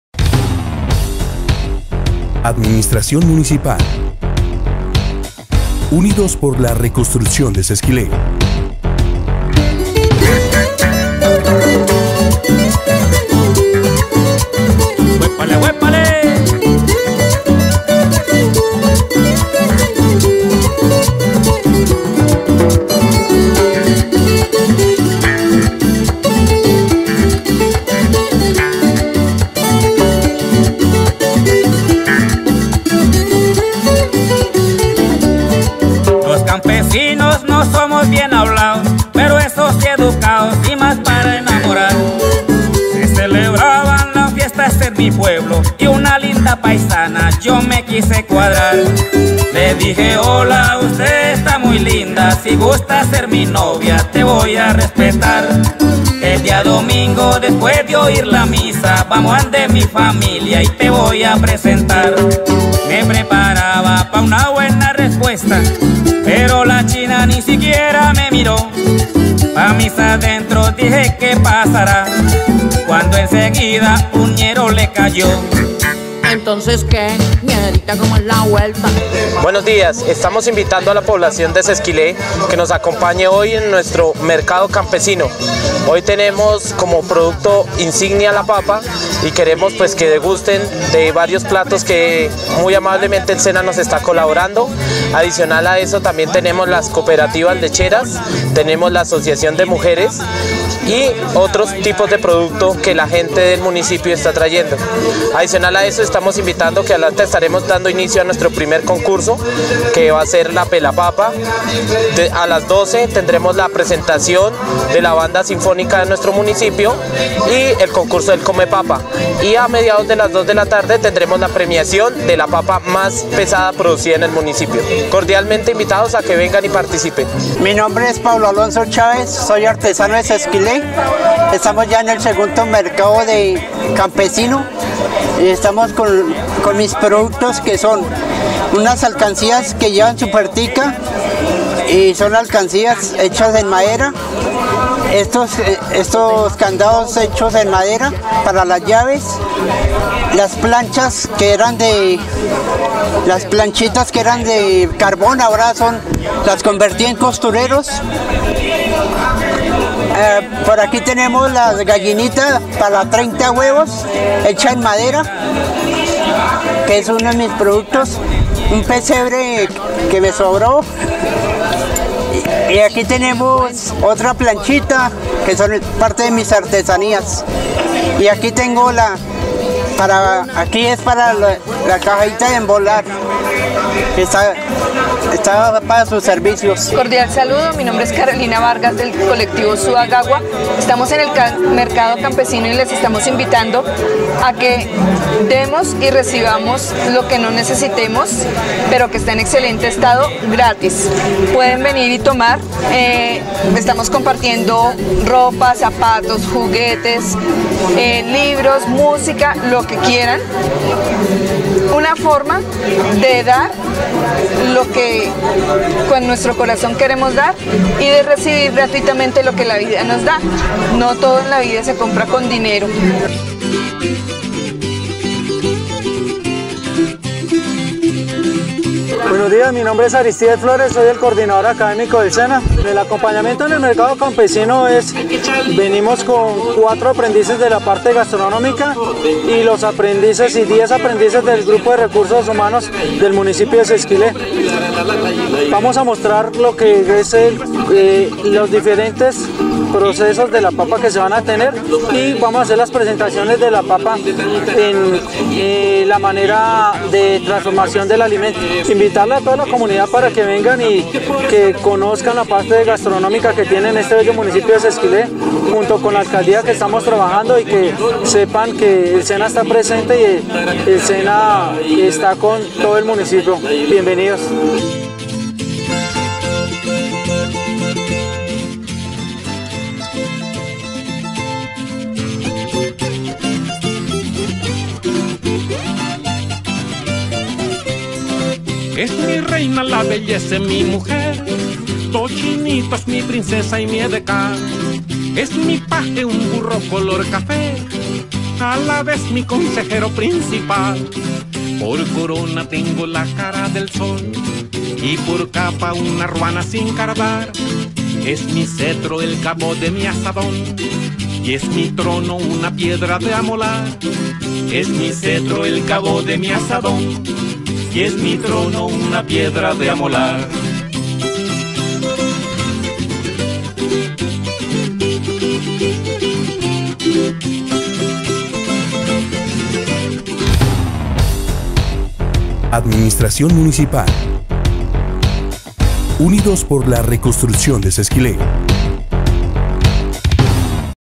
Segundo Mercado Campesino Primeras horas
En las primeras horas de inicio del Segundo Mercado Campesino, Productores y la Secretaría de Desarrollo Rural, Emprendimiento y Medio Ambiente y el SENA extienden invitación a la Comunidad